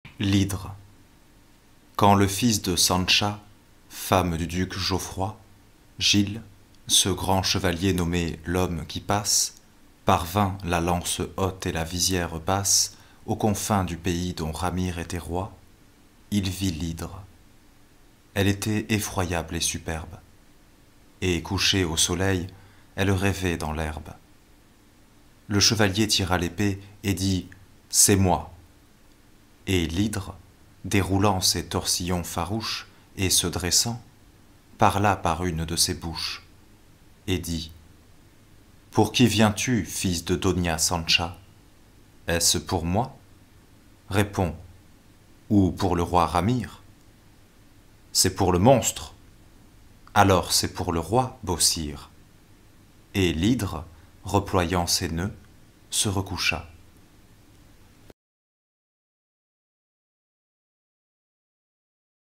Victor Hugo - L'Hydre - La légende des siècles
Depuis quand les créatures mythologiques font des vannes ?

Lu par Louis Milhau
bonne écoute